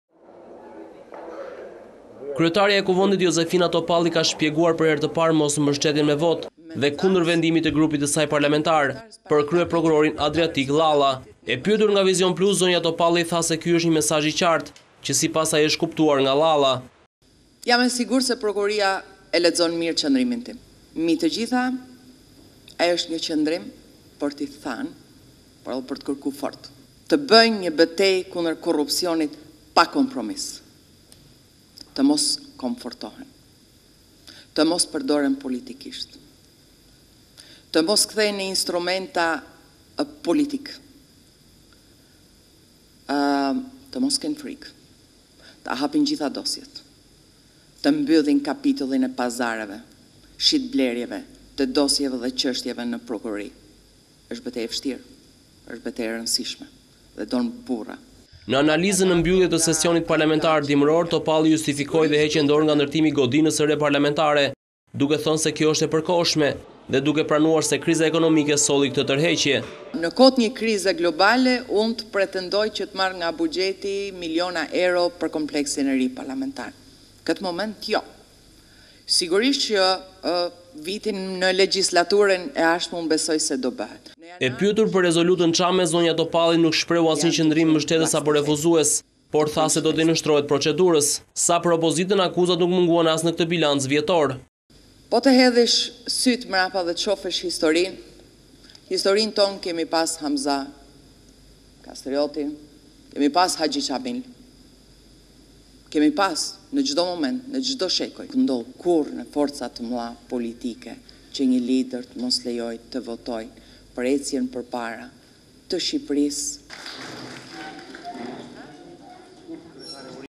Deklarata e Jozefina Topallit - Vizion Plus - News - Lajme
Kryetarja e Kuvendit Jozefina Topalli ka shpjeguar per here te pare mos mbështetjen me vote, dhe kundër vendimit te grupit te saj parlamentar per kryeprokurorin Adriatik Llalla.
E pyetur nga Vizion Plus, zonja Topalli tha se ky është nje mesazh i qarte, qe sipas saj është kuptuar nga Llalla. Ne analizën ne mbyllje te sesionit parlamentar dimëror, Topalli justifikoi edhe heqjen dore nga ndërtimi i ndërtesës se re parlamentare, duke thene se kjo eshte e përkohshme, dhe duke pranuar se kriza ekonomike solli këtë tërheqje. E pyetur per rezolutën çame, zonja Topalli nuk shprehu asnjë qëndrim mbështetës apo refuzues, por tha se do t'i nënshtrohet procedurës.

Speaker Topalli explained for the first time not supporting vote, and the decision of its parliamentary group chief Adriatik Llalla.
Asked by Vizion Plus, Topalli said that this is a clear message that as it is understood by Lala. In the analysis at the end of the winter parliamentary session, Topalli justified the waiver by the construction of a new parliamentary building, saying that this is temporary, and recognizing that the economic crisis brought this attraction. Asked about the resolution Cham Topalli expressed no supporting or rejecting attitude, but said he would undergo the procedure.